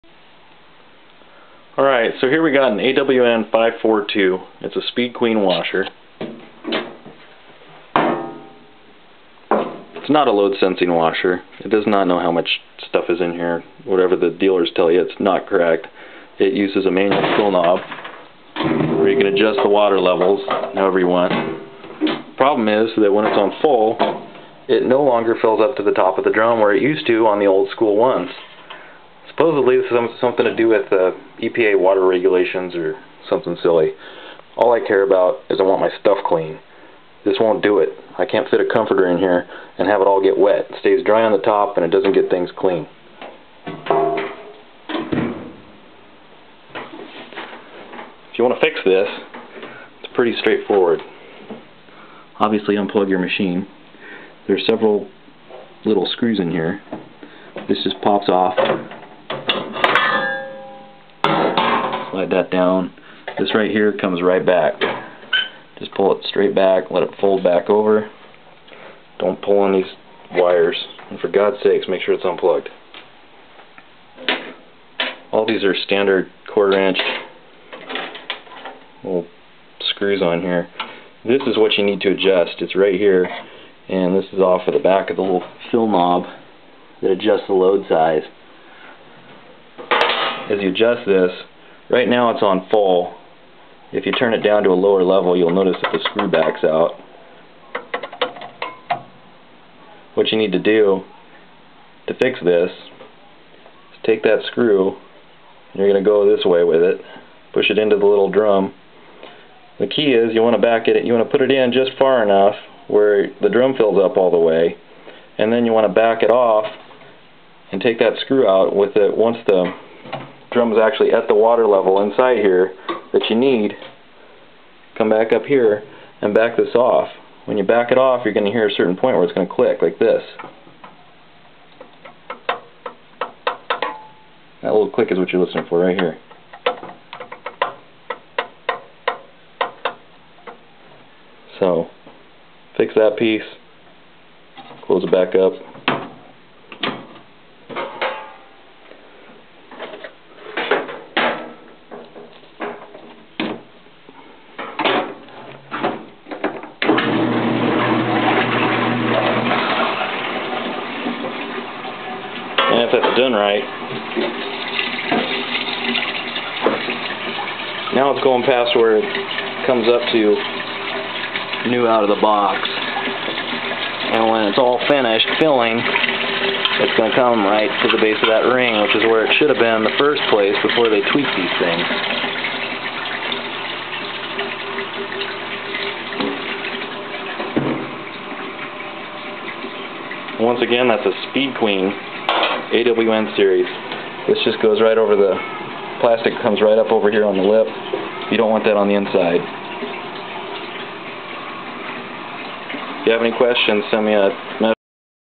Speed queen Clothes Washer water level problem and how to fix it
Speed queen washing machines are built well but they do not fill to the top with water anymore like they used to, instead you get a weak 70% fill that won't let you get big loads like comforters clean, the top stays dry and dirty.  I've heard this short fill has something to do with new government regulations placed on the manufacturers, like they did for our toilets in the 90's.  I don't know if that's true or not, I don't like this partial fill so I fixed it so that it works properly.   I couldn't find any documentation on how to fix this problem so here is documentation of my project for your entertainment.  This should not be considered an all inclusive how to guide.  This is what I did to fix mine.  You might break yours, or shock yourself silly doing this so if you aren't mechanically inclined don't even think of trying this.